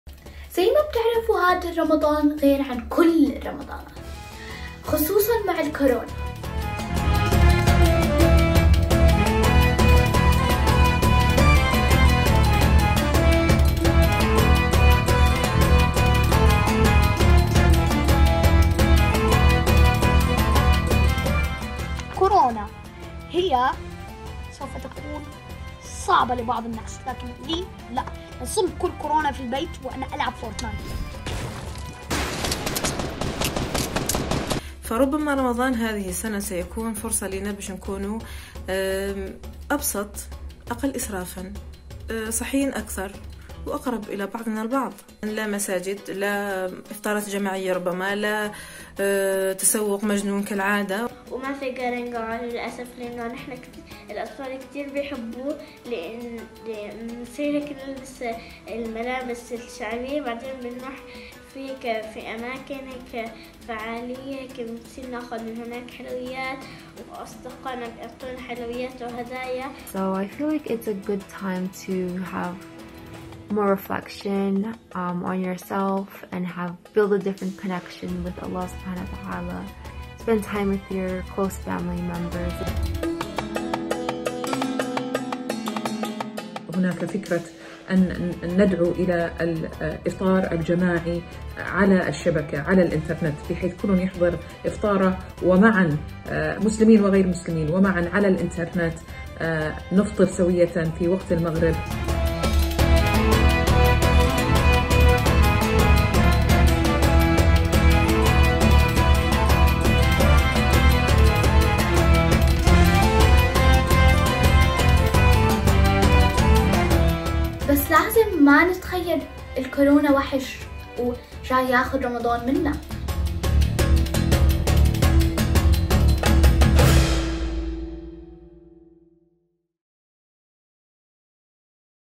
كيف سيكون رمضان 2020 مع كورونا؟
"رمضان هذا العام سيكون مختلفاً".. فما الذي سيتغير فيه بالنسبة إلى المسلمين؟ وكيف سيؤدون شعائرهم الدينية في ظل تغيرات كورونا؟ TRT عربي جمعت عدة آراء من دول مختلفة، تعرّفوها وشاركونا آراءكم.